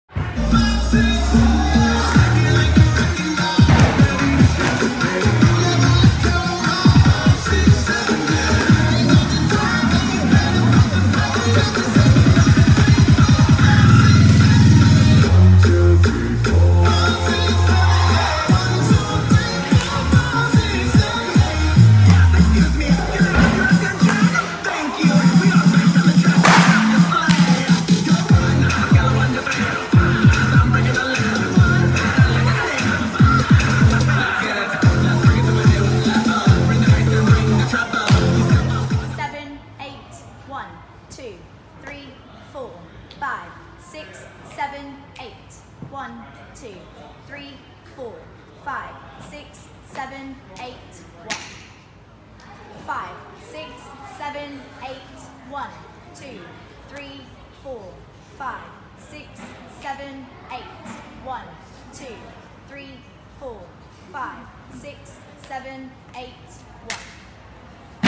ICE Athletics Prep Level 1 Trials Routine 2019
Allstar Elite Trials 2019.

Athletes new to cheer do not need to learn this routine unless they have a minimum of a back walkover.

Here is the Individual Trial routine for Prep level 1 tumble skills.

These are the preferred skills, however please just insert your best tumble skills.

Tumble 1: Standing
Tumble 2: Jump / standing combination
Tumble 3: Running

Prep level 1 tumbles include:
- forward roll
- backward roll
- cartwheel
- 1 handed cartwheel
- back bend
- kick over